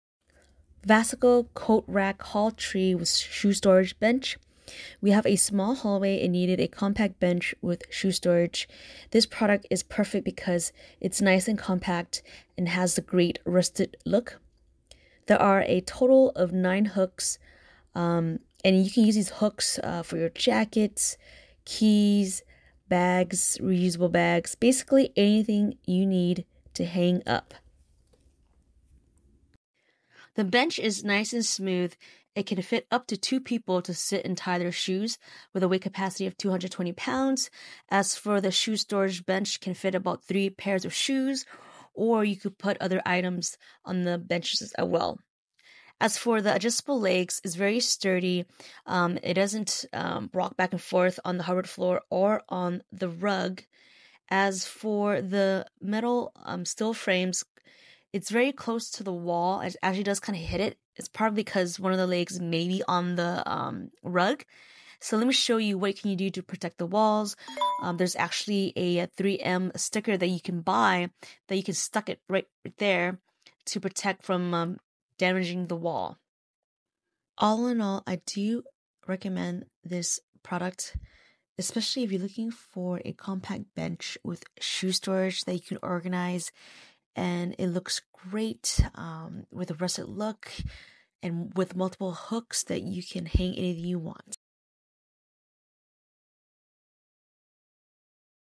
Smart Storage Solutions: VASAGLE Coat Rack & Bench In-Depth
Say farewell to entryway chaos! Join me in this in-depth review as we dissect the features of the VASAGLE 3-in-1 Entryway Rack. Discover firsthand why this steel-framed beauty seamlessly combines practicality and style.

🤲 Hands-On Demo: Witness why I've become a devoted fan of this 3-in-1 Entryway Rack —practical, stylish, and a perfect fit for any space!
🤔 Real Talk: No fluff, just my honest thoughts and insights on why the VASAGLE Entryway Rack is a game-changer.

🌟 Explore More Finds: Dive into my treasure trove of Amazon Product Reviews at my storefront Uncover your next favorite thing!